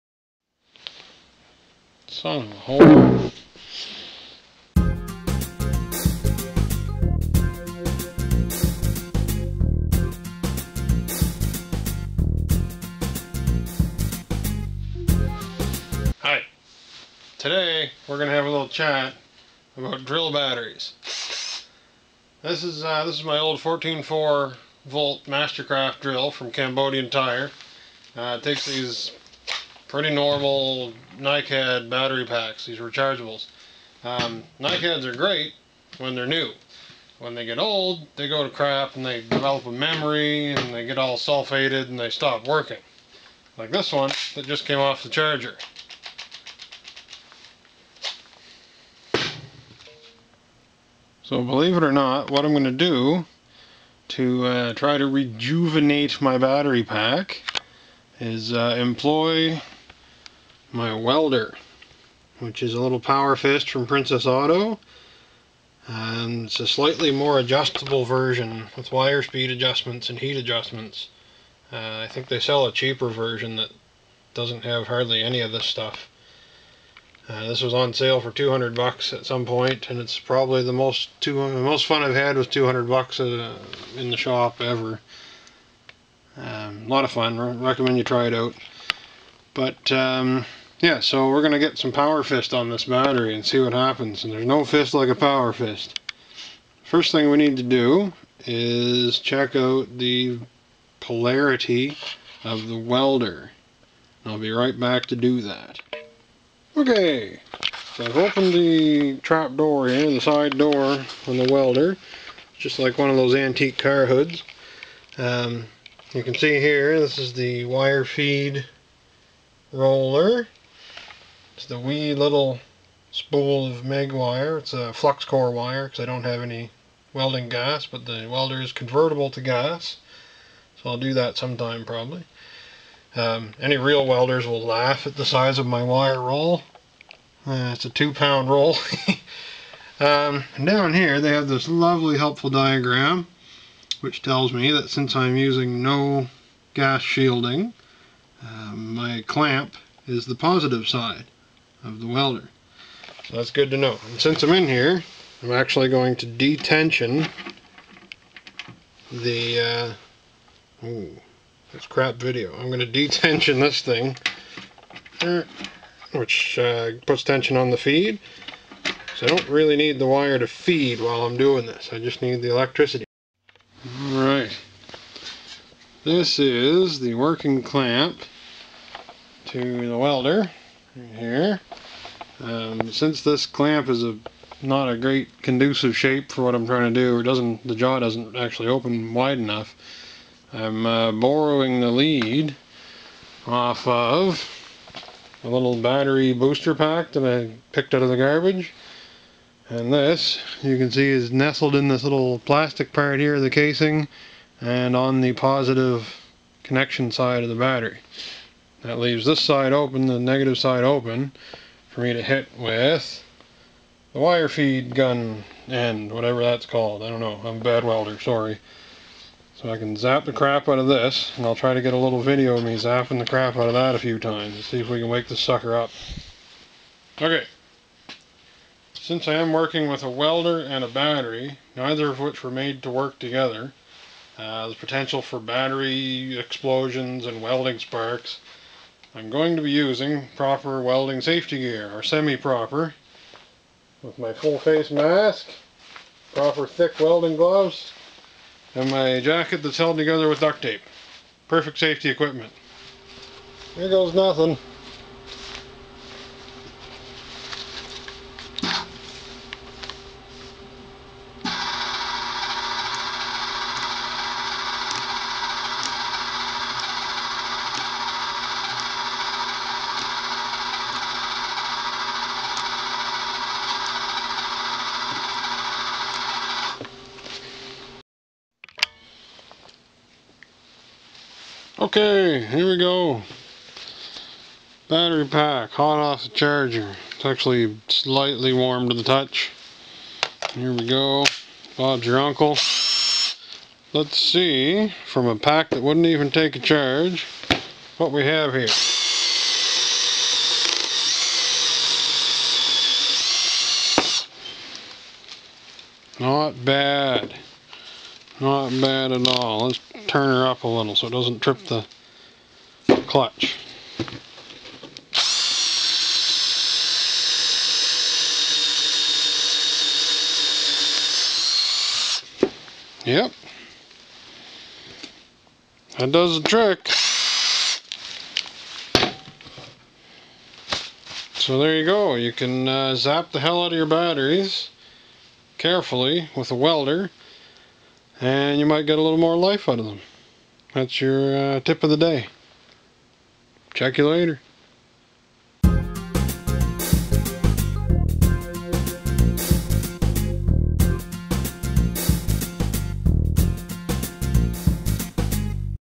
How To fix: Zap a Dead Cordless Drill Battery (nicad repair tutorial)
Got an awesome cordless drill with dead nicad battery packs and don't want to throw it out? Maybe a welder is the answer you're looking for...
IF YOU ARE GOING TO TRY THIS WITH YOUR OWN BATTERY, YOU DO SO AT YOUR OWN RISK.

for making his awesome music available for idiots like me to use.